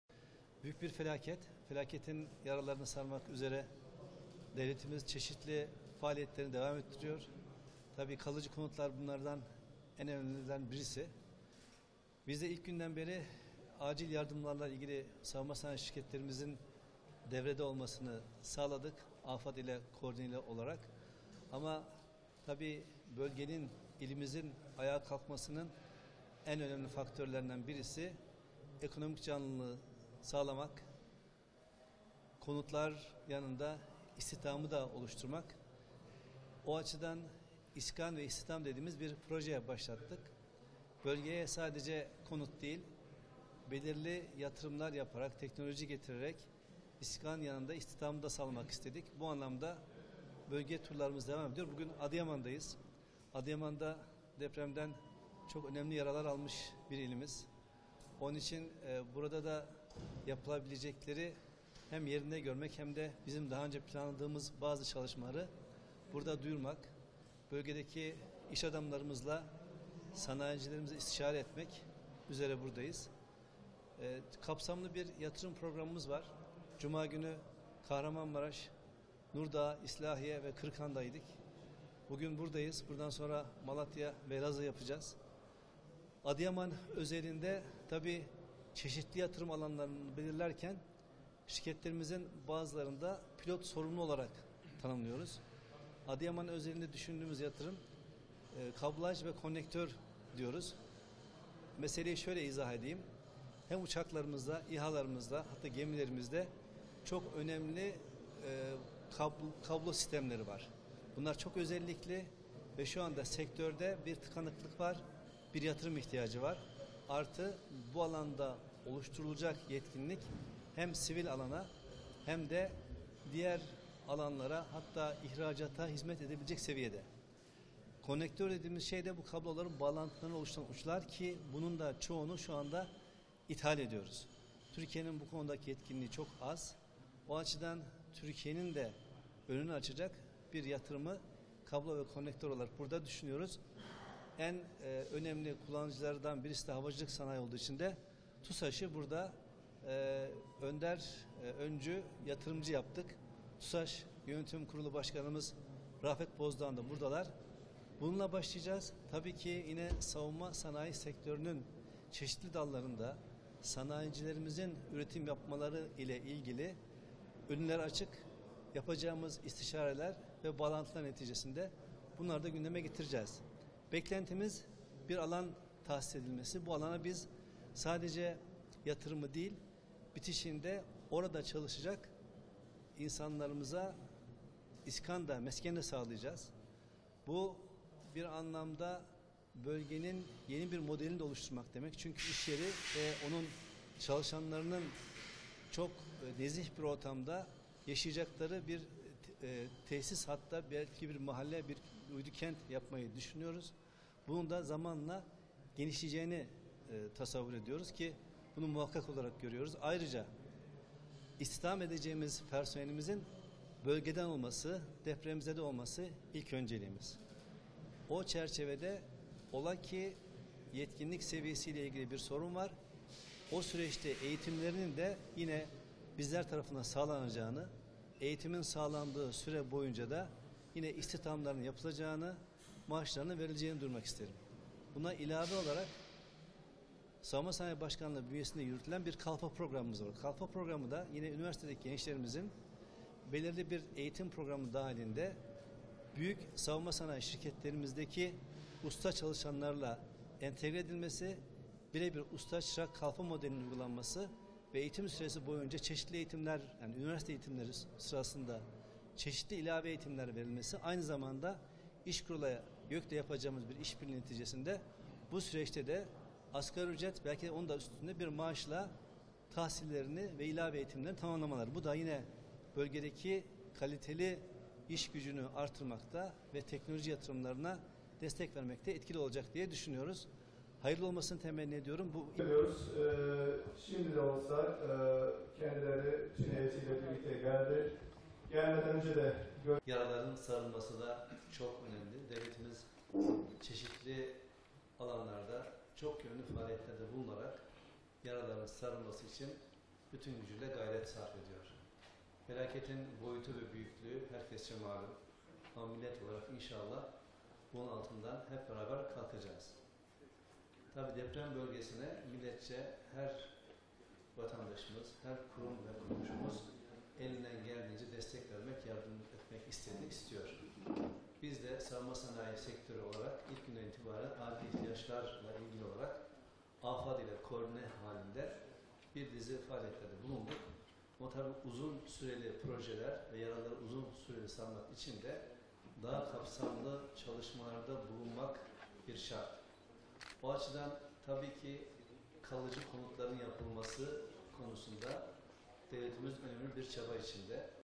Savunma Sanayii Başkanı Demir Adıyaman'a Kablaj ve Konnektör Tesisi Kuracağız
SAVUNMA Sanayii Başkanı İsmail Demir, depremin vurduğu Adıyaman’a TUSAŞ önderliğinde kablaj ve konnektör tesisi kuracaklarını söyledi.
Savunma Sanayii Başkanı İsmail Demir, çeşitli temaslarda bulunmak üzere Adıyaman’a geldi. Valiliği ziyaret eden Demir, burada gazetecilerin sorularını yanıtladı. Adıyaman’a tesis kurulacağını belirten Demir, uçaklarda, İHA ve gemilerde çok önemli kablo sistemleri olduğunu vurgulayarak, şöyle konuştu:
“Bölgede AFAD ile koordineli olarak ama tabii bölgenin, ilimizin ayağa kalkmasının en önemli faktörlerden birisi ekonomik canlılığı sağlamak, konutlar yanında istihdamını da oluşturmak. O açıdan istihdam projesi başlattık. Bölgeye sadece konut değil, belirli yatırımlar yaparak, teknoloji getirerek istihdam sağlamak istedik. Bu anlamda bölge turlarımız devam ediyor. Bugün Adıyaman’dayız, kapsamlı bir yatırım projemiz var. Adıyaman özelinde tabii çeşitli yatırım alanları belirlerken, şirketlerimizin bazılarını da pilot sorumlu olarak tanımlıyoruz. Adıyaman özeline düşündüğümüz yatırım, kablaj ve konnektör tesisi. Meseleyi şöyle izah edeyim; hem uçaklarımızda, İHA’larımızda hem de gemilerimizde çok önemli kablo sistemleri var, bunlar çok önemli ve şu anda sektörde bir tıkanıklık var, bir yatırım ihtiyacı var. Artı bu alanda oluşturulacak etkinlik hem sivil alana hem de diğer alanlara ihracata hizmet edebilecek seviyede konnektör dediğimiz şey de bu kabloların bağlantılarını oluştururken bunun çoğunu da ithal ediyoruz. Türkiye’nin bu konudaki etkinliği çok az. Bu açıdan Türkiye’nin de önünü açacak kablaj ve konnektör yatırımını burada düşünüyoruz.  En önemli kullanıcı havacılık sanayii olduğu için de TUSAŞ’ı burada önder, öncü yatırımcı yaptık. Ayrıca istihdam edeceğimiz personelimizin bölgeden olması önceliğimiz.”
Yaraların sarılması noktasında gayret sarf ettiklerini de kaydeden Demir, “Yaraların sarılması çok önemli. Devletimiz çeşitli alanlarda çok yönlü faaliyetlerde bulunarak yaralarımızın sarılması için bütün gücüyle gayret sarf ediyor. Felaketin boyutu ve büyüklüğü inşallah bunun altından hep beraber kalkacağız. Tabii deprem bölgesinde her vatandaşımız her kurum ve kuruluşumuz elinden geldiği kadar destek vermek istiyor. Biz de savunma sanayii sektörü olarak ihtiyaçlar olarak AFAD ile koordineli halinde bir dizi faaliyetlerde bulunduk. Ama uzun süreli projeler, daha kapsamlı çalışmalarda bulunmak şart. O açıdan tabii ki kalıcı konutların yapılması konusunda devletimiz önemli bir çaba içerisindedir” diye konuştu.
İsmail Demir, daha sonra organize sanayi bölgesinde iş inşalarıyla bir araya gelerek kurulması planlanan tesise ilişkin istişarede bulundu.